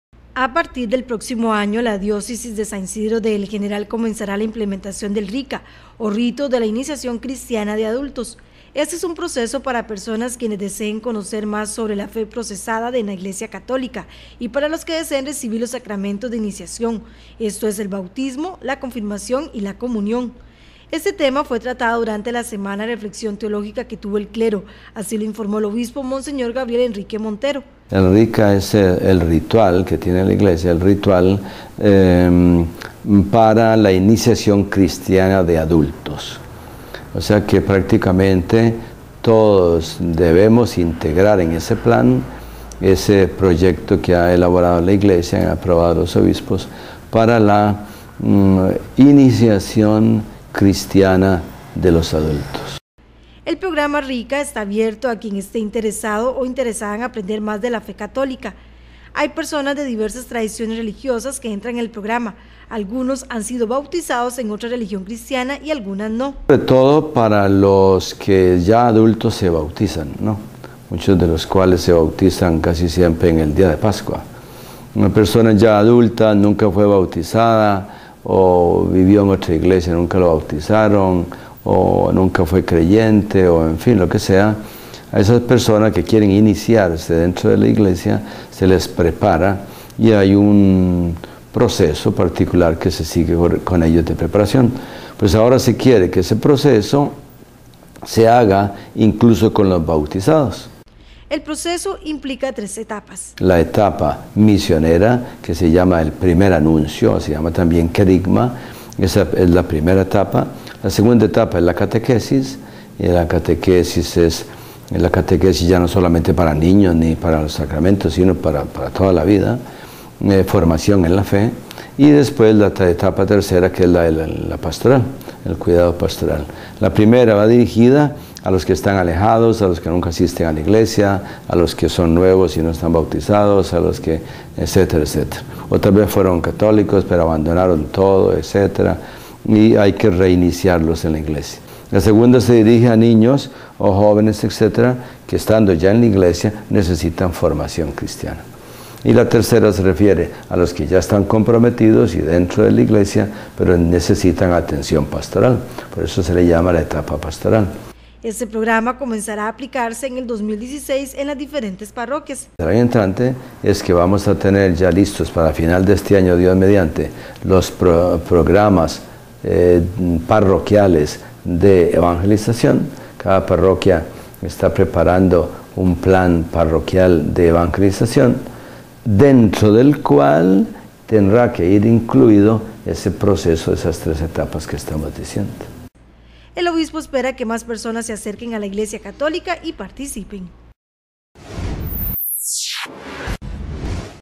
Diócesis comenzará en el 2017 el Rito de la Iniciación Cristiana de Adultos
A partir del próximo año, la Diócesis de San Isidro de El General, comenzará la implementación de R.I.C.A. o Rito de la Iniciación Cristiana de Adultos.

Este es un proceso para personas que desean conocer más sobre la fe p